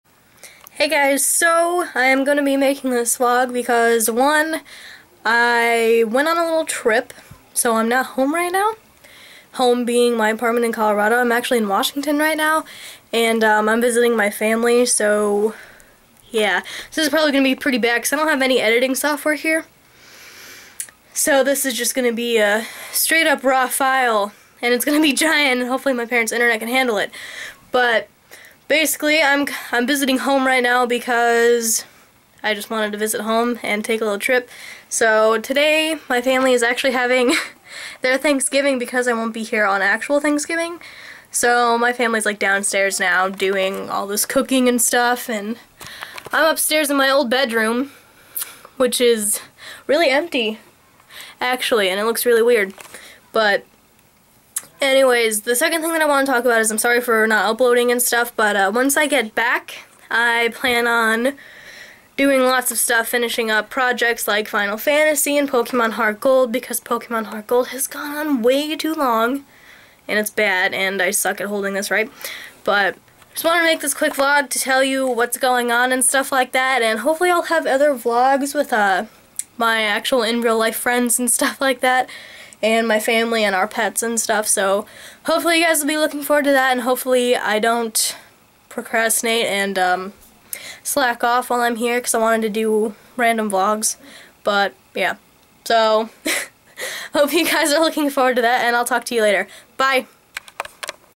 Back at Home for the Weekend
I decided to visit my parents for the weekend. Sorry for not uploading D;